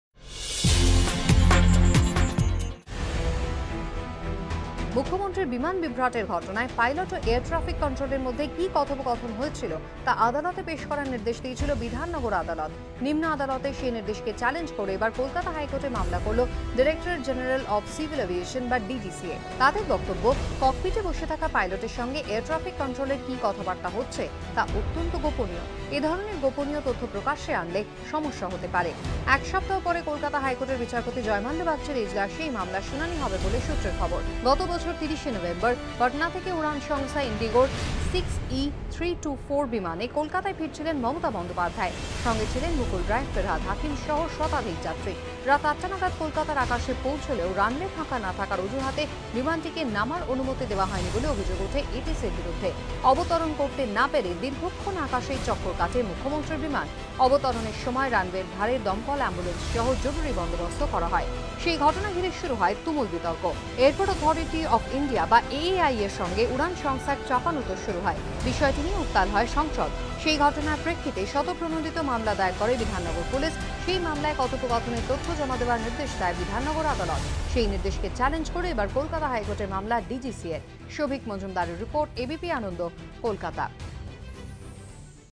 The DGCA today filed a petition to the Kolkata High Court on CM Mamata Banerjee's Flight
The DGCA today filed a petition to the Kolkata High Court on CM  Mamata Banerjee's Flight case